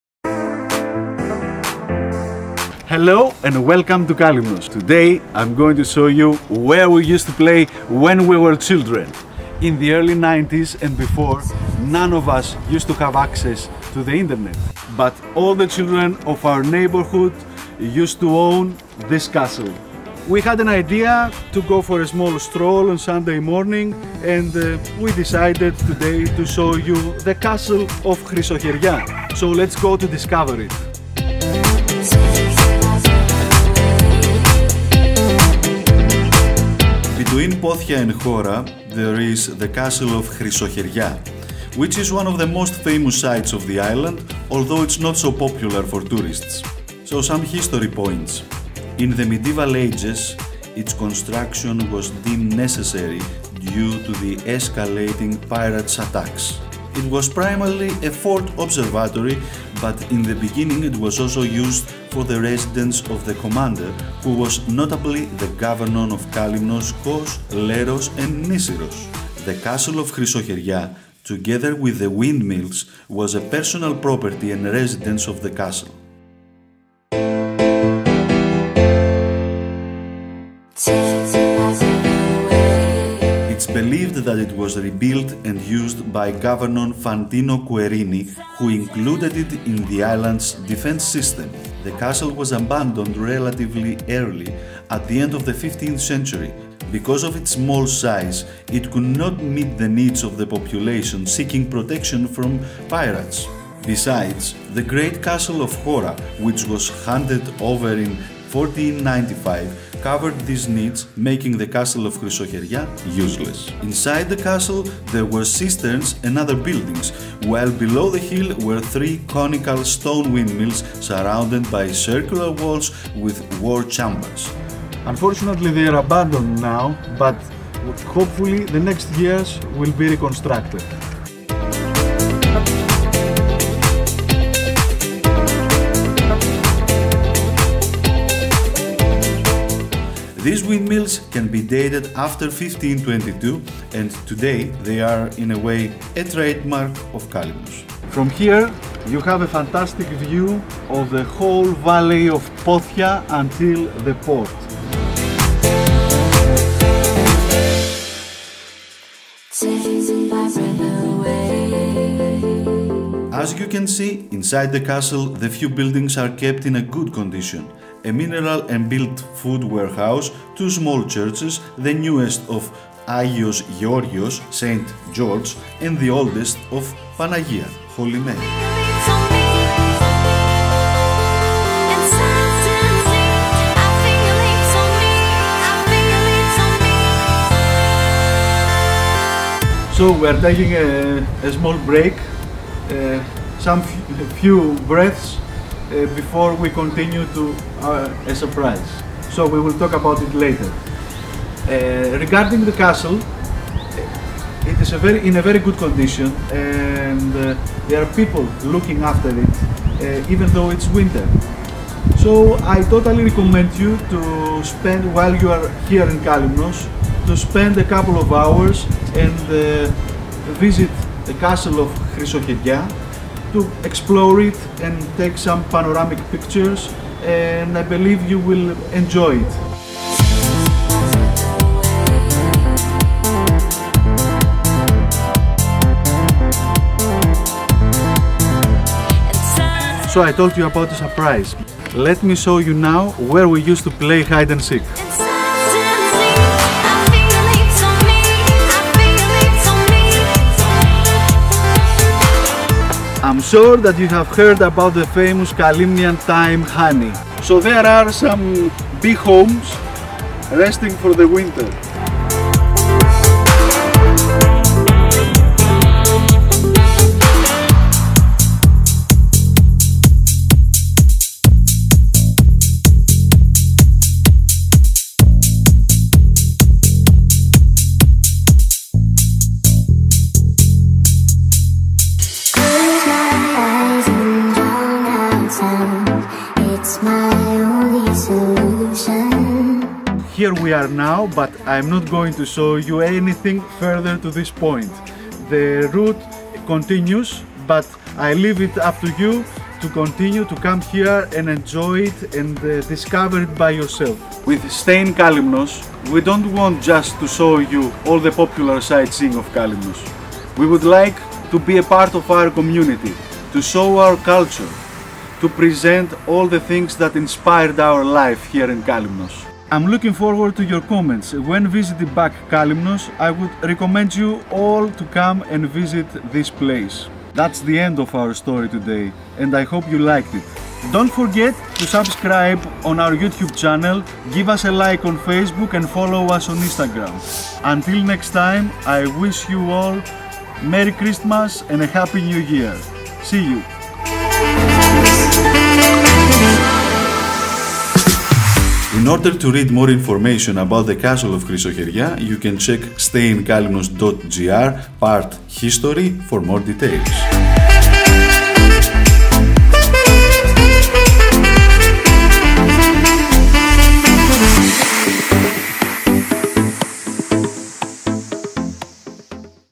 History of Kalymnos #1: The Castle of Chrysocheria
A few days before we say "Goodbye" to the 2020 we decided to go for a small excursion in the Castle of Chrysoheria which is easily aproachable by vehicle and on foot.

Let's see some historical facts about the Castle:
"In the middle of the route between Chora and Pothia you can easily spot the Pera Kastro or “Chrysocheria Castle”, which is one of the most famous sights of the island. Built at a strategic point, on a natural hill, it provided the possibility of supervising Pothia (the current capital and main port) and communicating with Chora (the Castle).

The castle is known by the names “Chrysocheria“, “Pera Kastro” and “Chrysoserza“.

In the medieval ages, its construction was deemed necessary due to the escalating pirates attacks. It was primarily a fort-observatory, but in the beginning it was also used for the residence of the commander, who was notably the governor of Kalymnos, Kos, Leros and Nisyros.
The castle of Chrysocheria, together with the mills, was a personal property and residence of the castle. It is believed that it was rebuilt and used by Governor Fantino Querini (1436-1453), who included it in the island’s defense system. After a accusation by Querini and a trial that took place in Rhodes, the inhabitants of the island were forced in 1445 to take part in the work of a castle. These works may be related to the castle of Chrysocheria, because in it there is a coat of arms of Querini, who would obviously repair it for his residence.

In our website except from informations about Kalymnos you can book your holidays through our booking engine choosing between different kinds of accommodation.

Looking forward to read your own unique experience in Chrysoheria Castle!!

We wish everyone to be healthy and safe !!
Best wishes for the 2021 !!